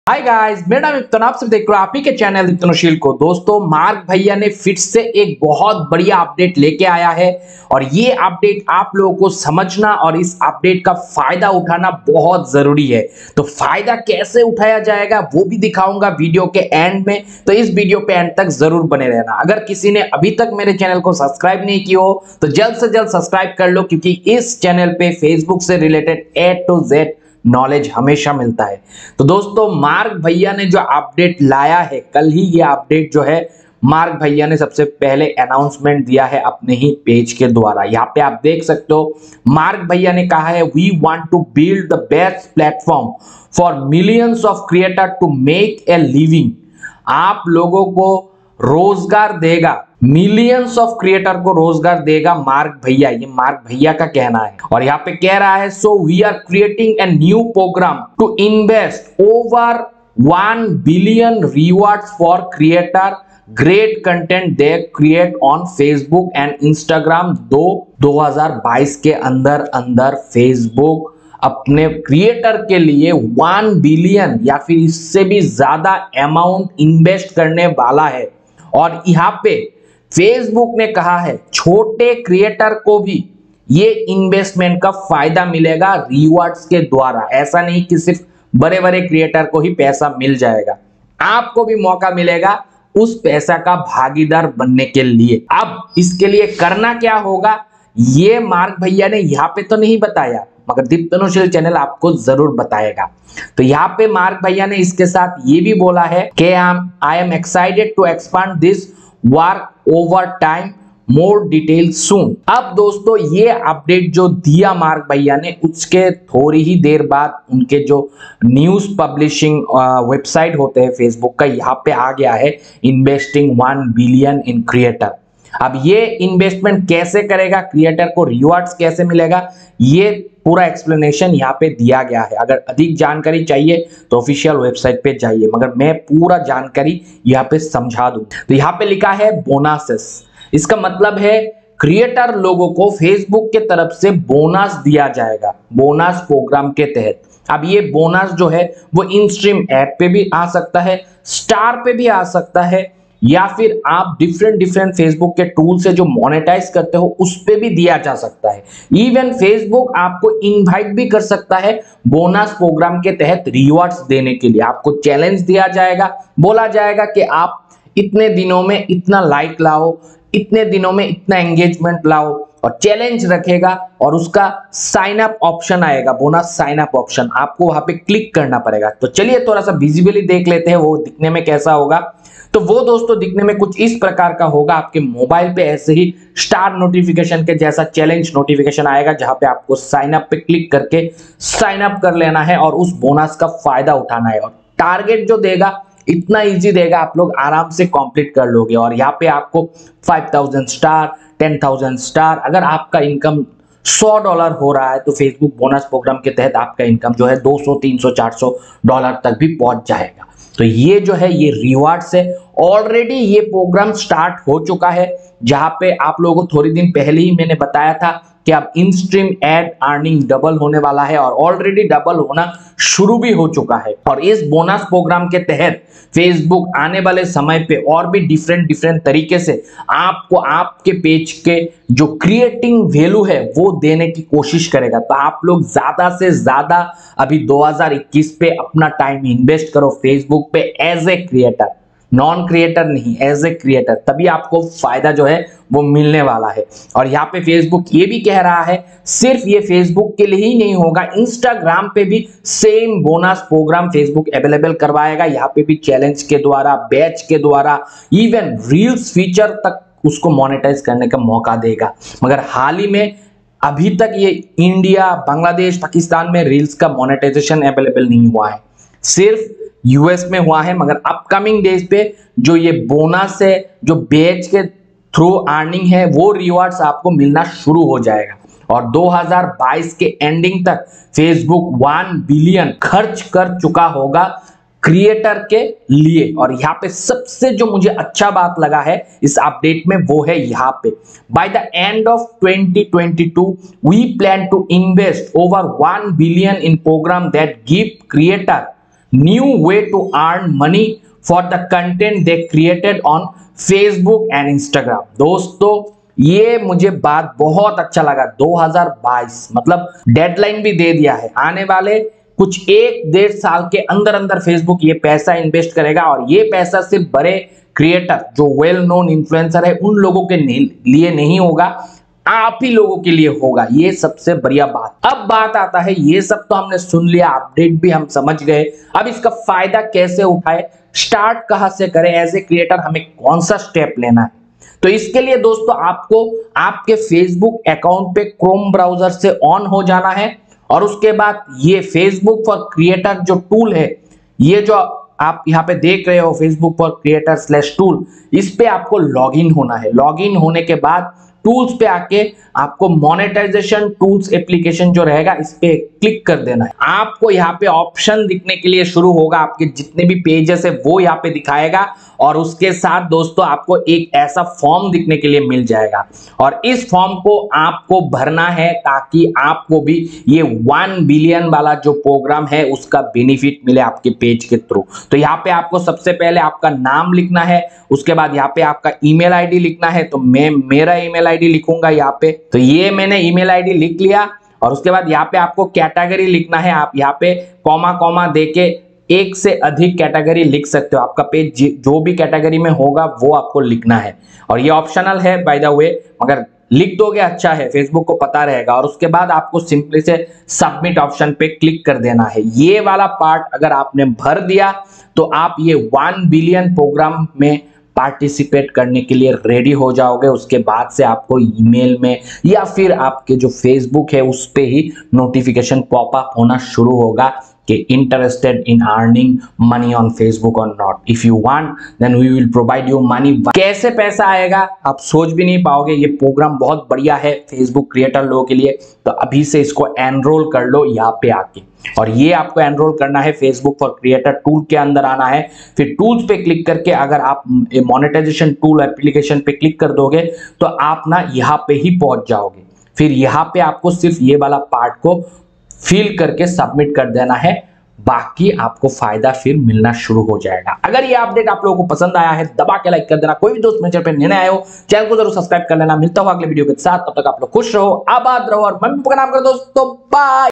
Interesting in Earning Money on Facebook? Investing $1 Billion in Creators | Facebook New Update
Interesting in Earning Money on Facebook? Investing $1 Billion in Creators | Facebook New Update // By the end of 2022, we plan to invest over $1 billion in programs that give creators new ways to earn money for the content they create on Facebook and Instagram. The first bonus opportunities on Facebook, available by invitation now, help creators earn more while learning how to create content that can be monetized.